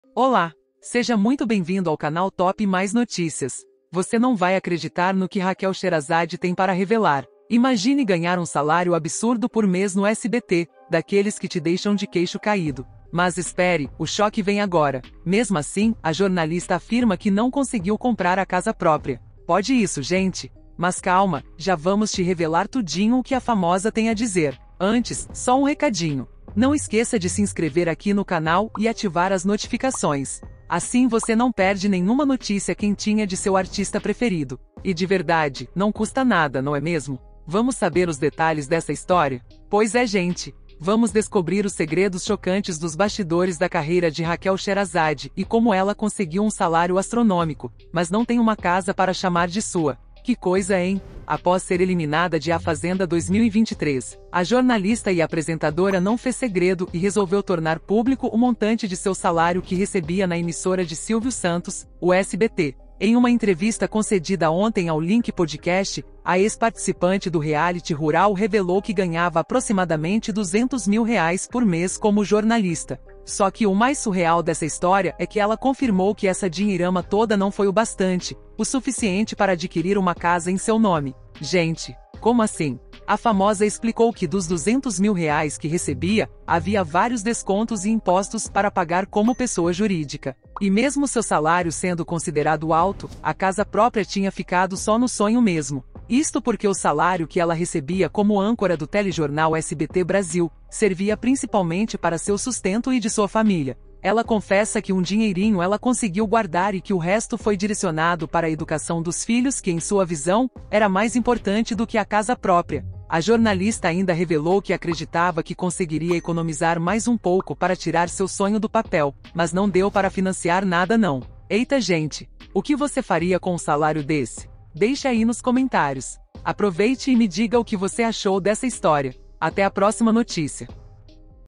RACHEL SHEHERAZADE abre o jogo, revela SALÁRIO EXORBITANTE no SBT e diz que NÃO TEM CASA PRÓPRIA
Créditos de imagens: Instagram e Internet
Créditos de Artes no Canal: @savioferraz